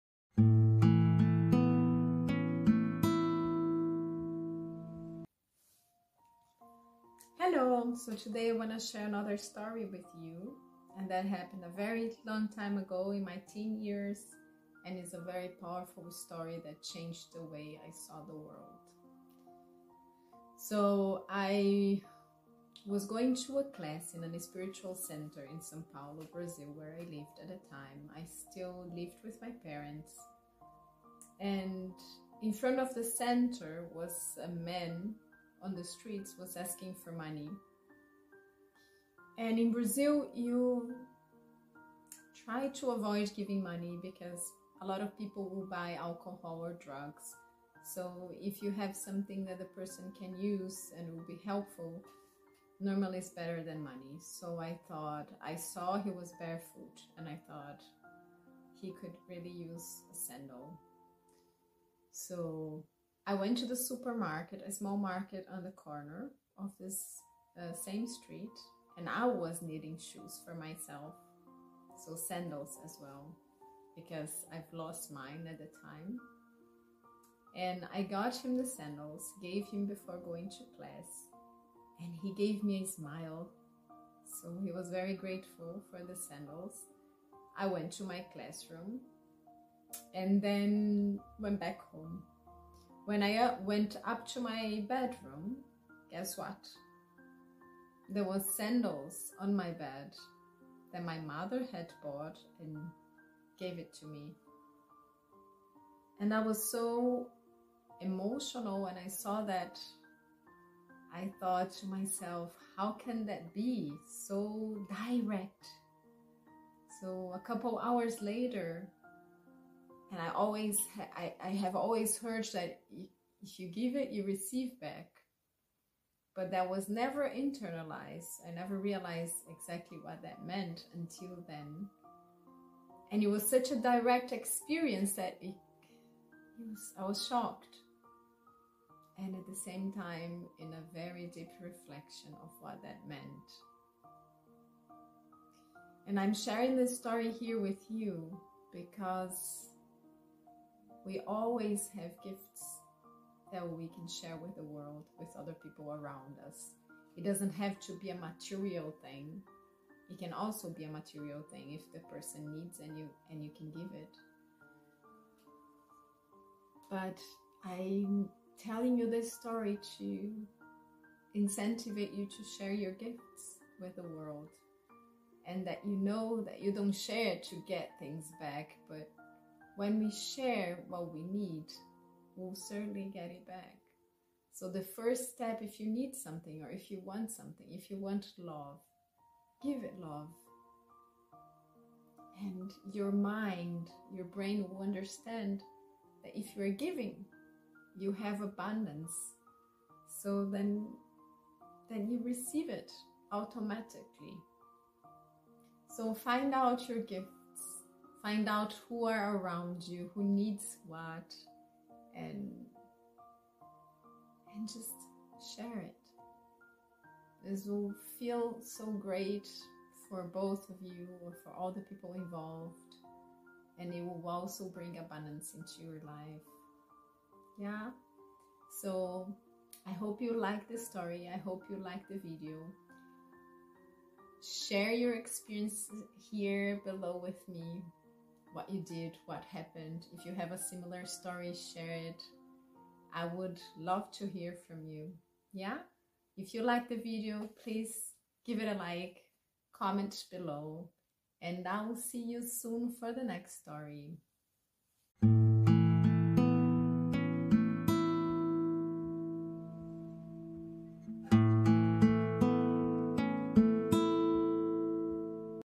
The Sandals
Here comes the story...

Today I'm going to tell you a story that made a big impression on me and taught me in practice that it's by giving that you receive.

Everything we do and emanate comes back to us. This is one of the most important spiritual principles to understand in life, in my opinion.

So let's reflect: how are you treating the people and the world around you, how are you acting and what thoughts are you emanating? Is this consistent with what you want to receive?

Be the change you want to see in the world! Gandhi.

Have fun listening and share it with friends or people who could benefit from the story.

If you want to see a movie about this topic, it's one of the most moving movies I've ever seen. Here are the links to find it on Amazon, but you can also find it on Youtube or Apple. Pay it forward is the name of the movie:

Namaste!

Narjara Thamiz